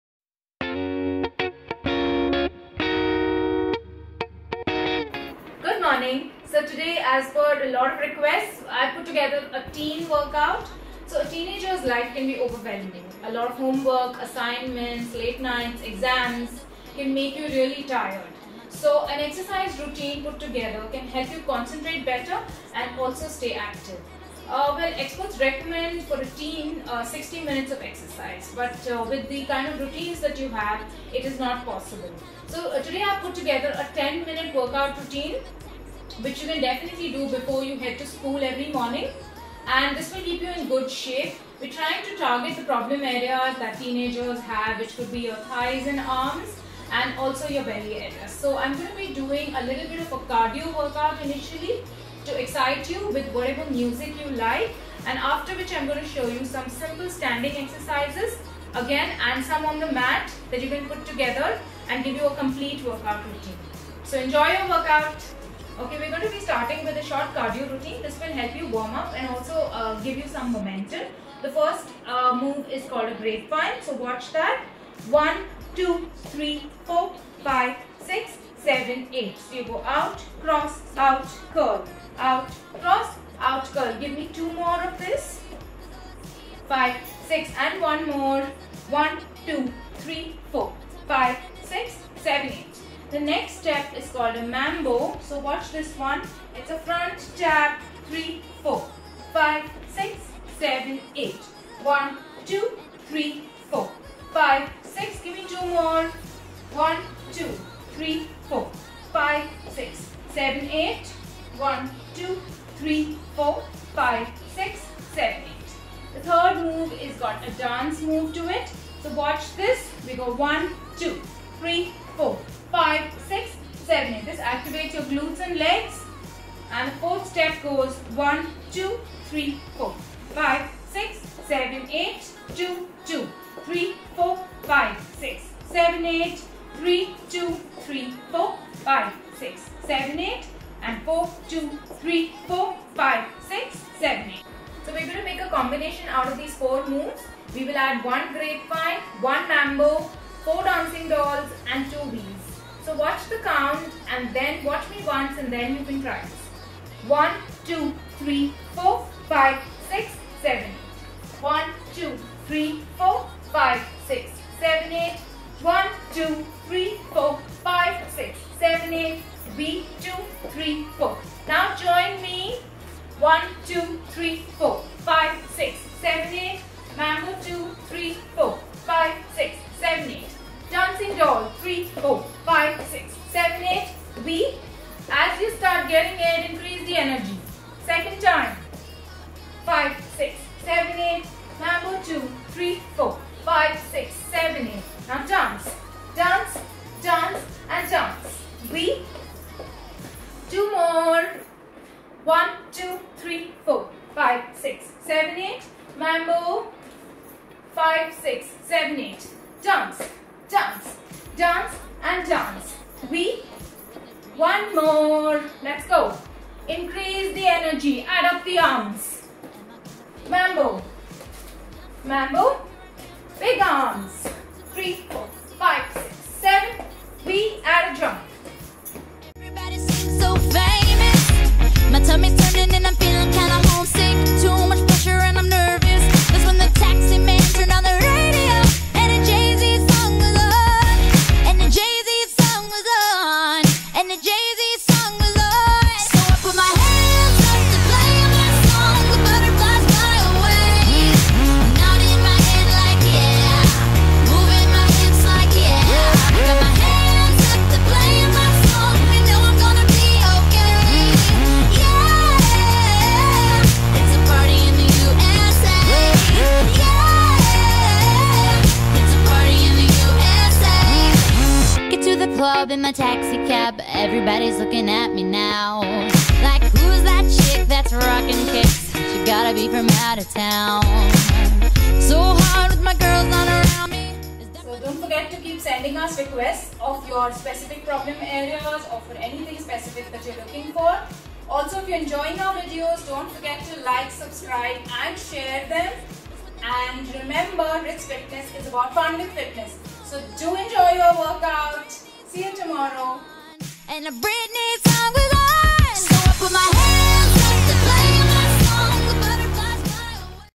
Ritz@home workout series Day 16 : Teens Special Workout
This is a workout for teenagers leading overwhelming lives with study, assignments, exams etc.
This workout is excellent to energize them and increase concentration levels.
A 15 minutes regular workout routine can keep teenagers active the whole day